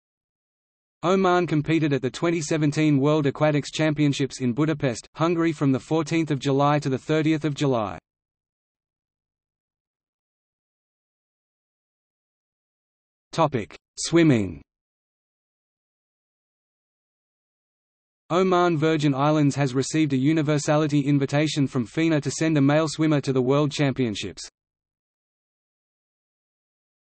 Oman at the 2017 World Aquatics Championships | Wikipedia audio article
00:00:09 Swimming

- Socrates

SUMMARY
Oman competed at the 2017 World Aquatics Championships in Budapest, Hungary from 14 July to 30 July.